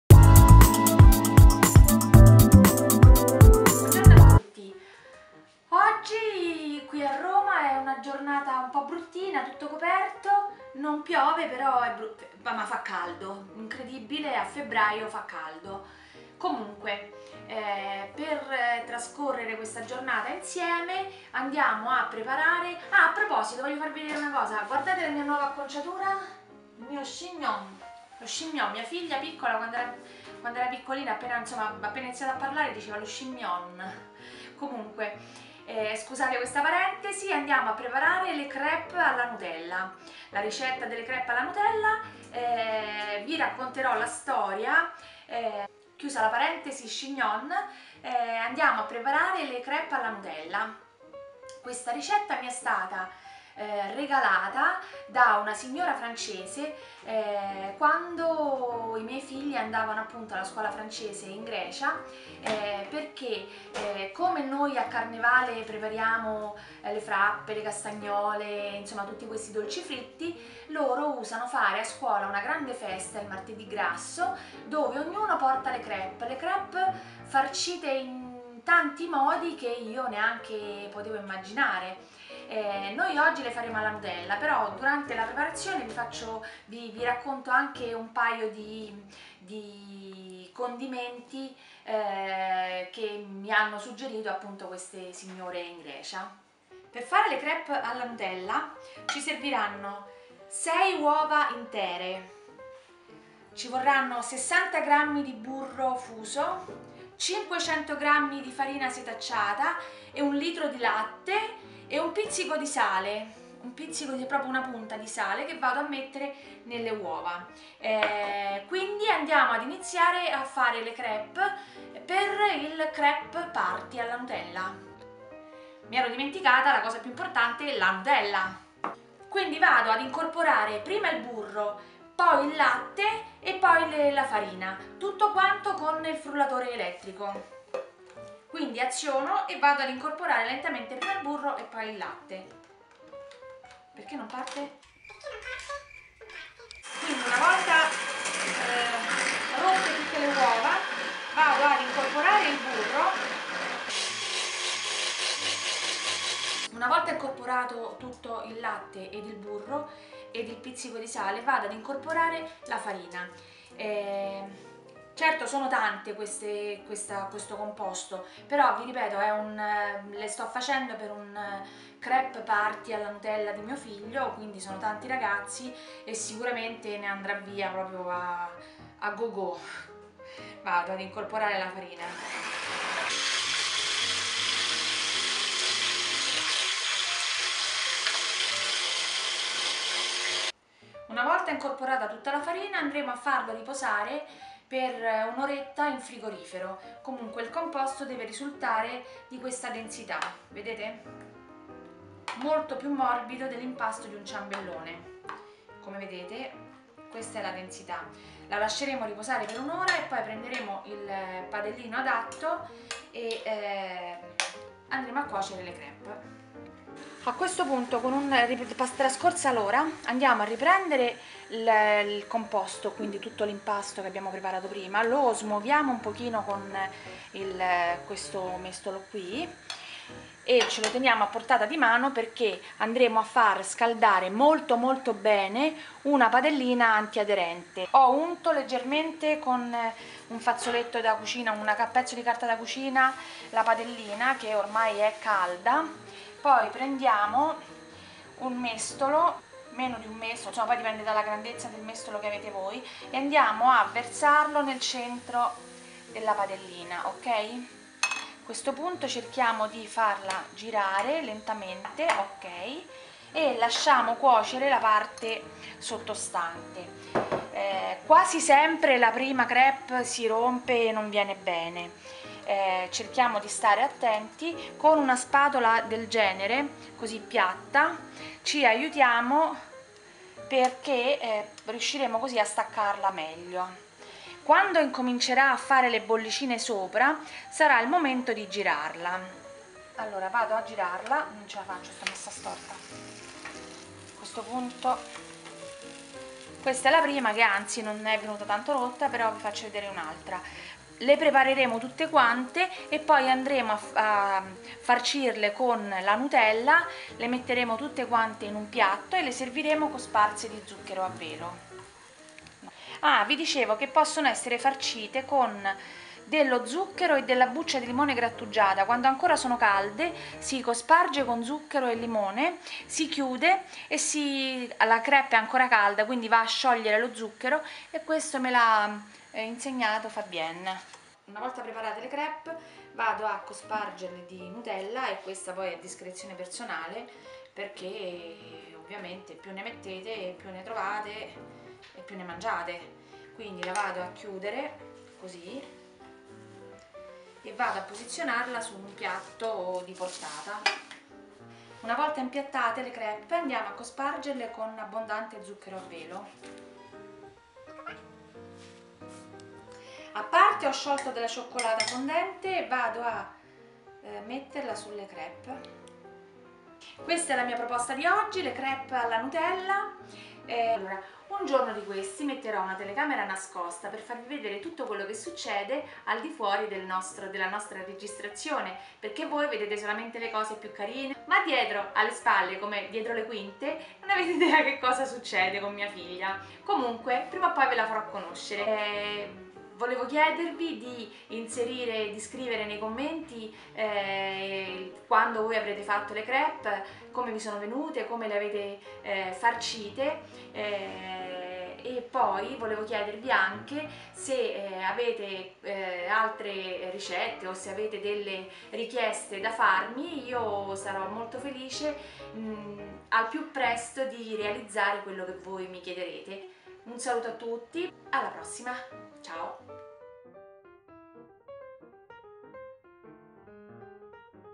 CREPES ALLA NUTELLA nutella | CRICUCINA
CREPES ALLA NUTELLA ricetta originale francese

Ho iniziato a cucinare le crepes, durante il mio soggiorno in Grecia.
A Salonicco ci sono dei piccoli locali, un po' come da noi le rosticcerie, dove preparano le crepes in tutti i modi possibili e non immaginabili per noi italiani.
Alla scuola francese abbiamo imparato che durante il carnevale si preparano per tradizione le crepes dolci, farcite in tanti modi ed ovviamente quelle alla nutella sono sempre le più' gradi, sopratutto dai bambini!
La mamma di un compagnetto di classe della mia terza figlia mi ha regalato la sua ricetta, io la voglio regalare a voi...
Gli ingredienti

6 uova intere
500 gr di farina
60 gr di burro fuso
1 lt di latte
un pizzico di sale fino

Per le crepes al cioccolato basterà togliere 60 gr di farina e aggiungere 60 gr di cacao amaro.

Considerate che con questo quantitativo verranno circa 40 crepes dipenderà' comunque dalla grandezza del vostro padellino, quindi potrete dimezzare tutti gl'ingredienti o raddoppiarli a seconda delle vostre esigenze. Potrete comunque provare prima meta' dose e regolarvi sulle vostre necessita', avete visto ci vuole pochissimo tempo per prepararle!
Buon appetito a tuttiiii!

📷SONO ANCHE SU INSTAGRAM: CRICUCINA